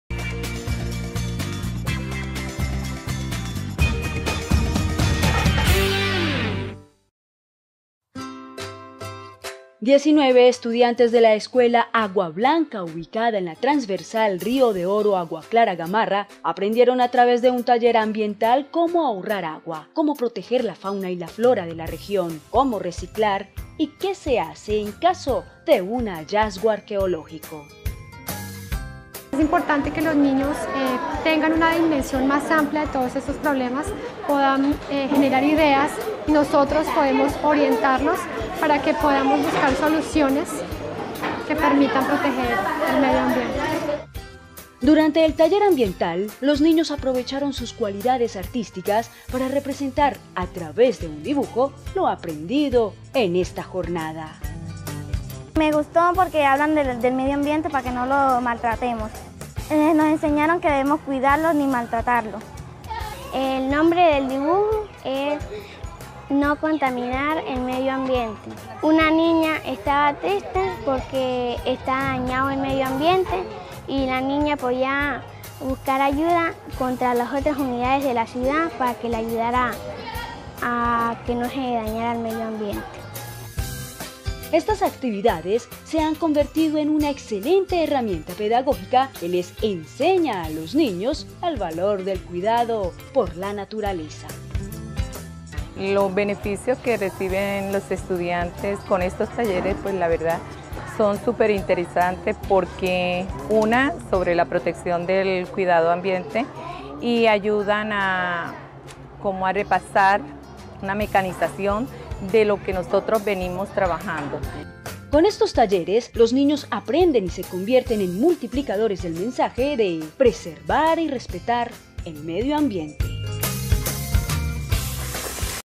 Estudiantes aprenden cómo cuidar el medio ambiente
A través de un taller realizado por CONSOL y la Concesionaria Ruta del Sol, estudiantes de la Institución Educativa Aguablanca aprendieron cómo cuidar el medio ambiente y la importancia de preservarlo.